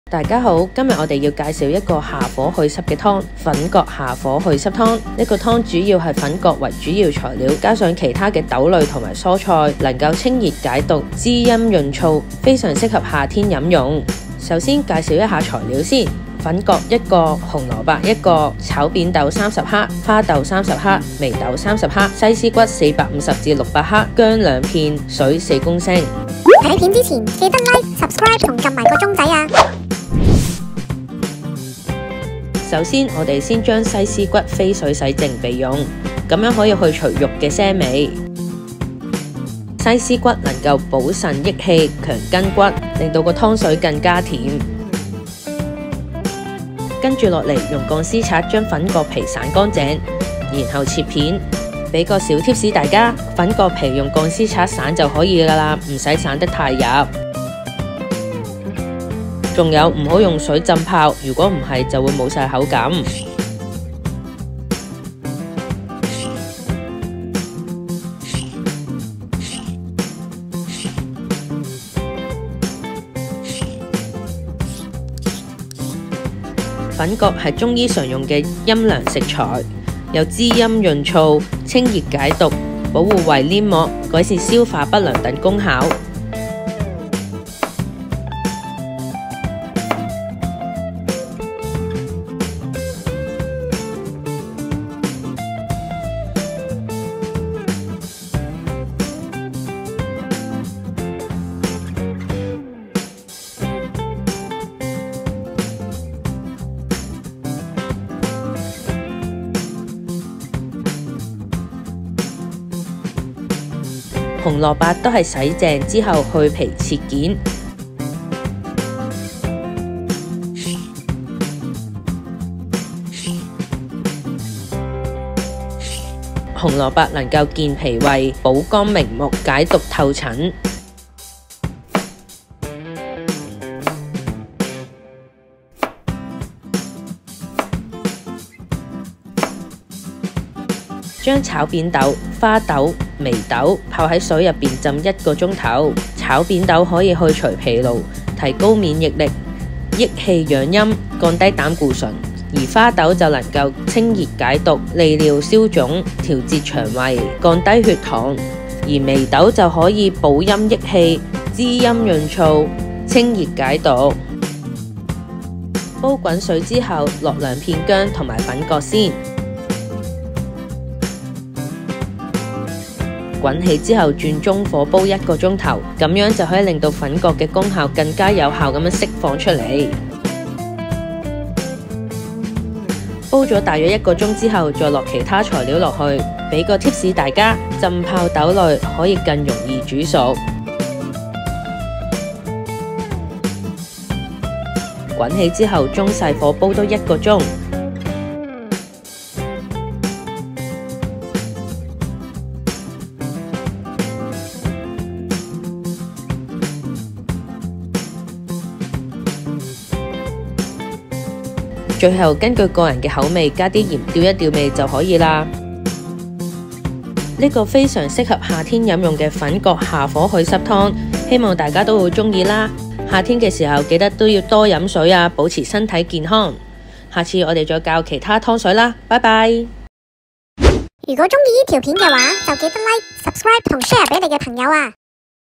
⬇️🔥 下火湯水 👍👍 粉葛下火袪濕湯 Kudzu soup for clearing heat and dampness｜下火｜祛濕｜滋陰｜解毒｜消除疲勞
夏天多雨水，加上出入冷氣間，總會覺但濕重疲勞，飲用以粉葛煲的湯水，就可以收下火、去濕、去疲勞的功效。這款湯的材料相對簡單，預備工夫很花就完成，煲湯話咁易。

功效
粉葛：滋陰潤燥，清熱解毒，保護胃黏膜，改善消化不良
紅蘿蔔：健脾胃、補肝明目、解毒透疹
炒扁豆：消除疲勞，提高免疫力，益氣養陰，降低膽固醇
花豆：清熱解毒，利尿消腫，調節腸胃，降低血糖
眉豆：補腎益氣，滋陰潤燥，清熱解毒
西施骨：補腎益氣，強筋骨，滋陰潤燥等功效，令湯水更甜

0:00 開始
0:30 處理材料
3:38 完成

請分享出去俾親朋好友 鍾意可以俾個like👍👍👍
歡迎大家分享食譜出去